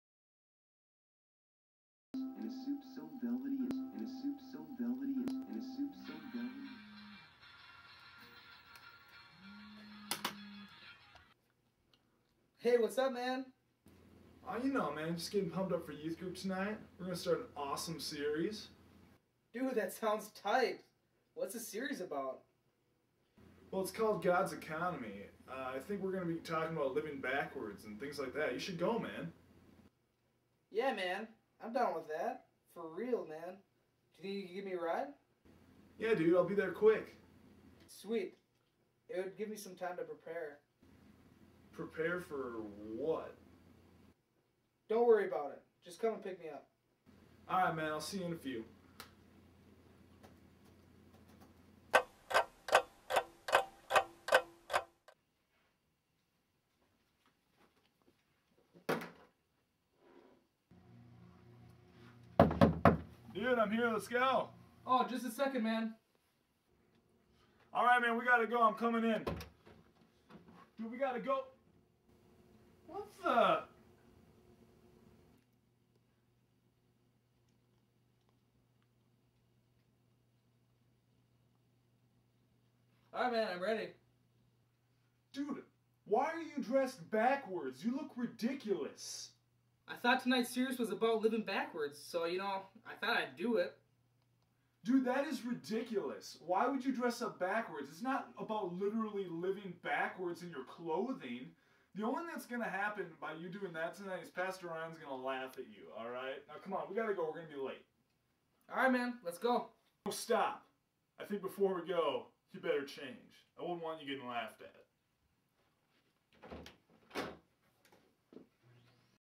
God's Economy: Living Backwards Promo
Promo for our new series "God's Economy: Living Backwards"